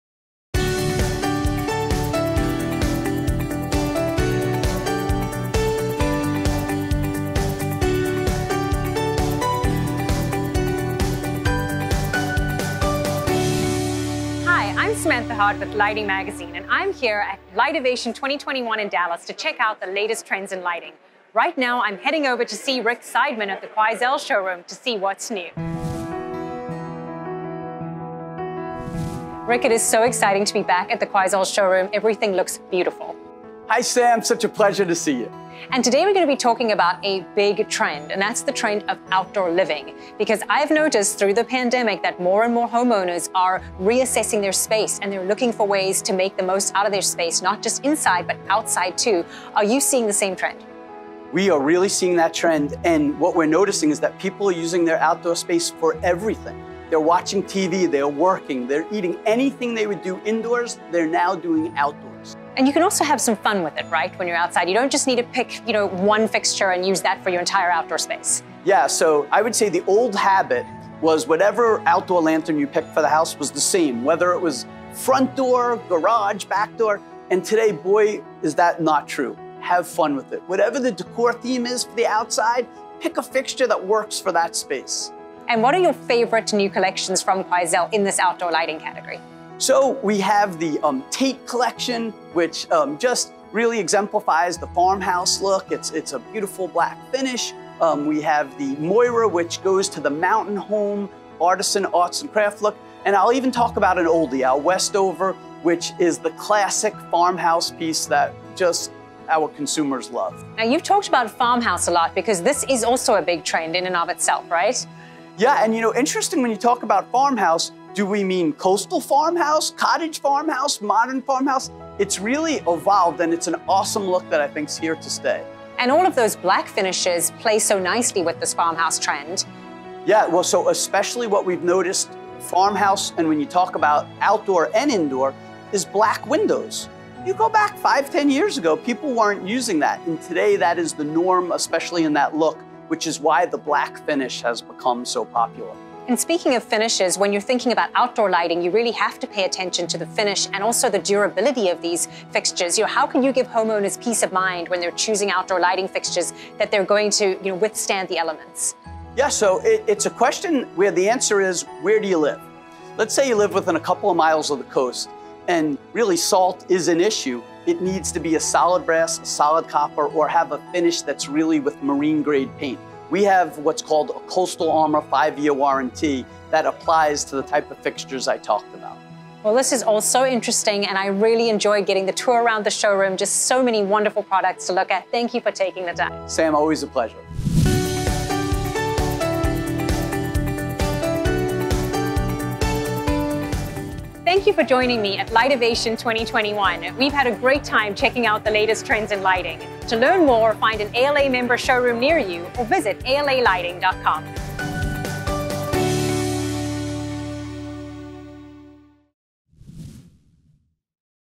Custom outdoor living with Quoizel
Lighting magazine Executive Editor Samantha Hart discusses the importance of multi-use outdoor living spaces with Quoizel, Inc. President and CEO Rick Seidman. Learn how you can customize your fixture choices to make the look and feel of your home shine.